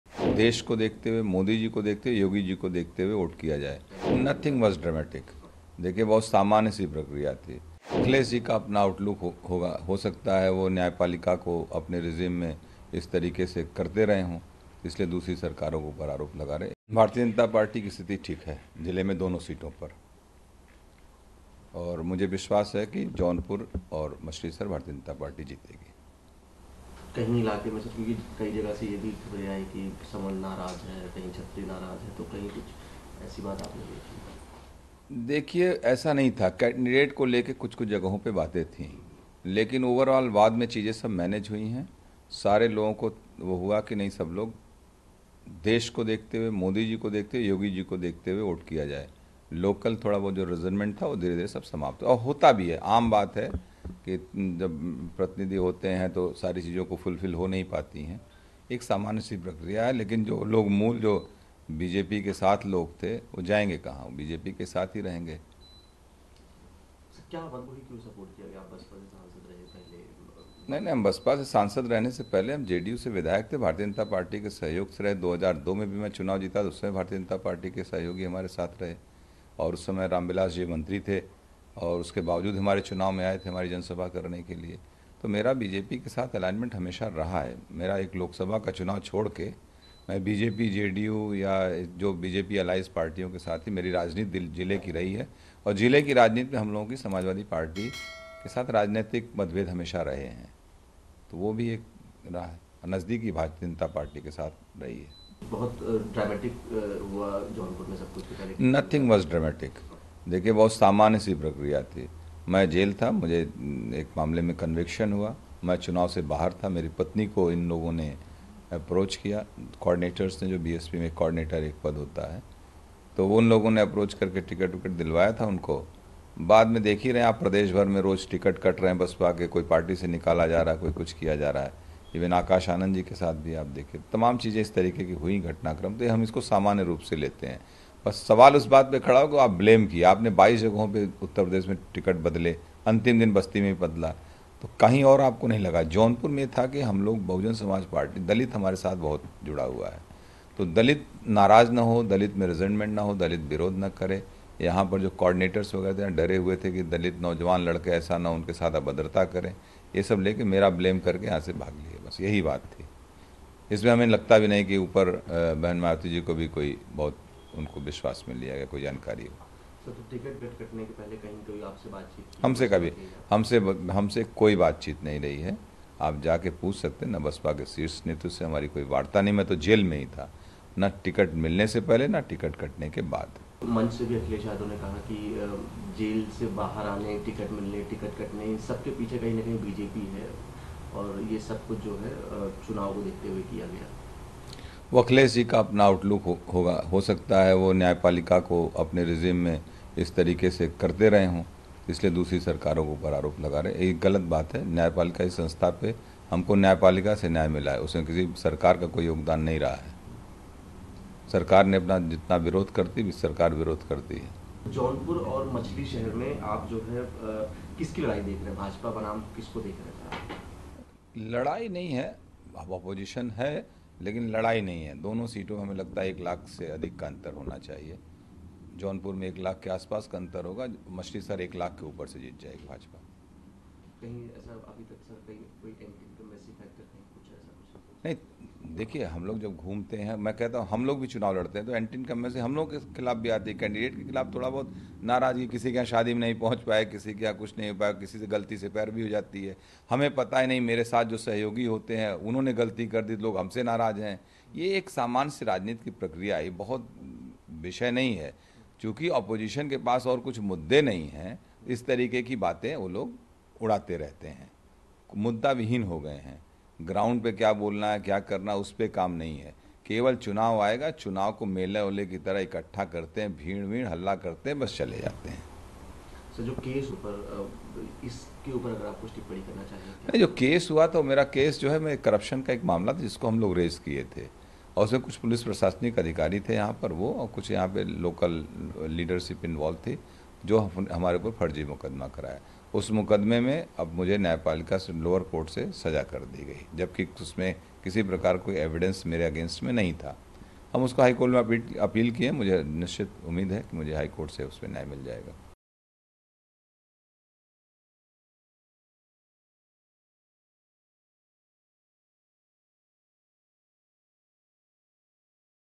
"कुछ भी नाटकीय नहीं था..." Jaunpur Lok Sabha में BJP के प्लस फैक्टर Dhananjay Singh ने क्या कहा?
जौनपुर (Jaunpur) के सियासी गलियारों में बीजेपी में अंदरूनी दिक्कत की बात सामने आ रही थी. जिस पर अब पूर्व सांसद धनंजय सिंह (Dhananjay Singh) की प्रतिक्रिया आयी है. वहीं, हाल ही में धनंजय सिंह की पत्नी श्रीकला सिंह को बसपा (BSP) से टिकट मिला था. धनंजय तब जेल में थे. नामांकन के ठीक पहले बसपा प्रमुख मायावती ने उनका टिकट काटकर श्याम सिंह यादव को दे दिया. जमानत पर बाहर आने के बाद उन्होंने भाजपा (BJP) के साथ देने का ऐलान कर दिया है. इस पर भी धनंजय सिंह ने बात की है.